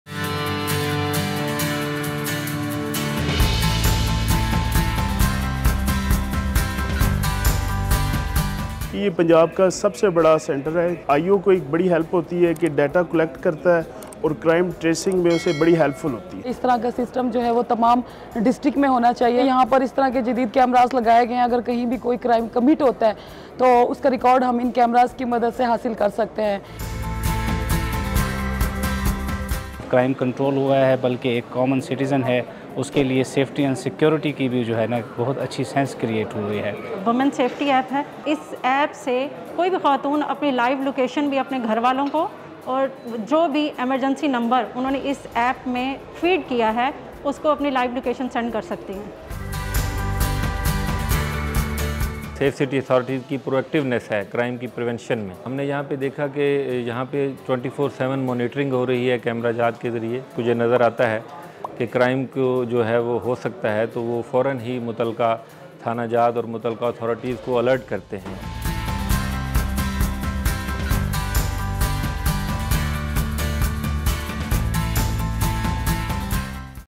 Undertraining Police Officers Visit Punjab Safe Cities Authority
A 40-member delegation of the promotion course was trained on various aspects of Safe Cities and modern policing. How are PSCA cameras assisting police operations and investigations? Find out in this special video

چوہنگ ٹریننگ سینٹر میں زیر تربیت پولیس افسران کا سیف سٹیز اتھارٹی کا تربیتی دورہ
پروموشن کورس کے 40 رکنی وفد کو سیف سٹیز کے مختلف شعبہ جات اور ماڈرن پولیسنگ بارے تربیت دی گئی۔ سیف سٹی کیمرے پولیس آپریشنز اور انویسٹی گیشن میں کس طرح معاونت کررہے ہیں؟ جانیے اس خصوصی ویڈیو میں